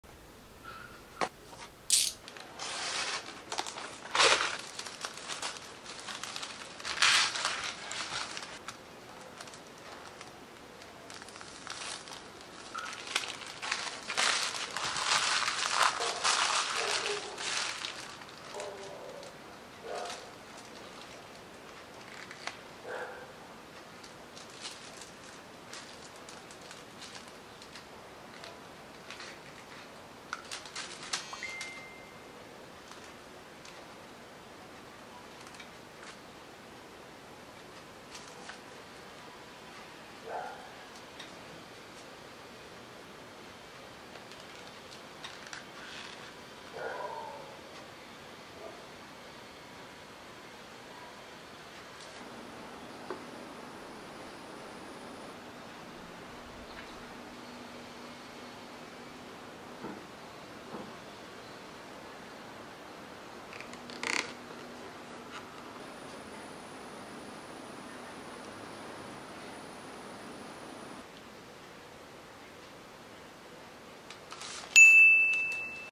Unboxing Lineal Design’s Version of Knarr Wallet By DS Leather Goods ASMR
Won Linael Design’s Knarr Wallet by DS Leather Goods and finally it arrived!
So i did a quick unboxing to show you how amazing this wallet build is!
🥰 hope you guys enjoy it also